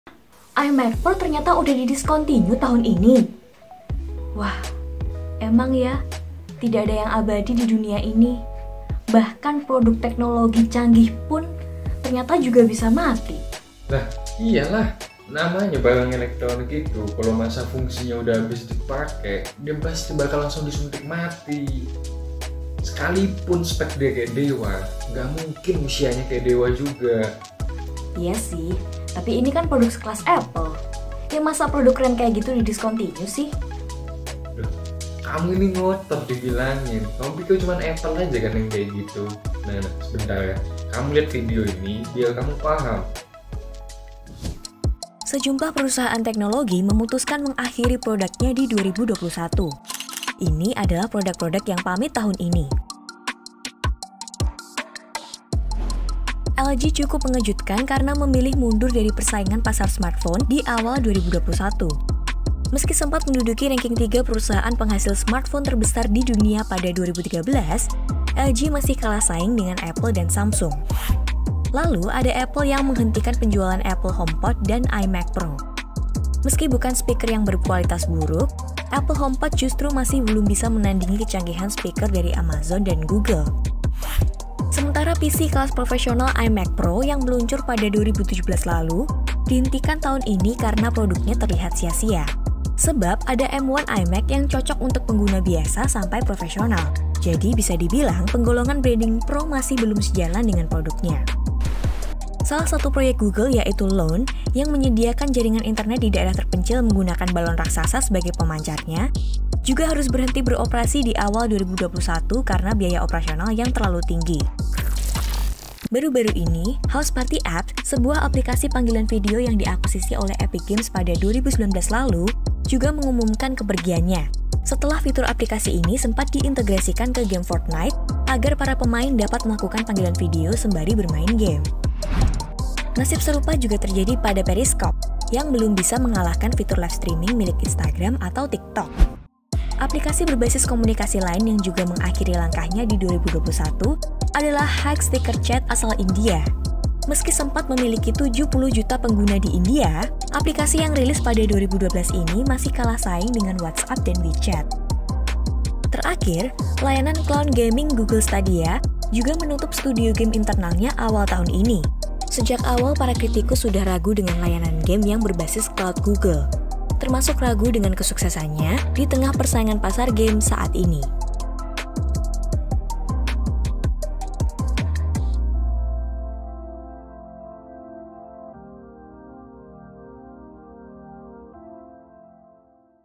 Meninggal pada 2021: iMac Pro dan Produk Tech Lain | Tech It Easy
Perubahan zaman terus bergulir, sejalan dengan inovasi yang terus dilakukan setiap harinya untuk menemukan teknologi yang lebih canggih. Teknologi terbaik di masanya mulai ditinggalkan dan tergantikan dengan yang lebih up to date, termasuk di 2021 ini.

Mau tahu produk-produk teknologi yang harus mati dan tergusur di tahun ini? Cek videonya, ya.

(Narasi)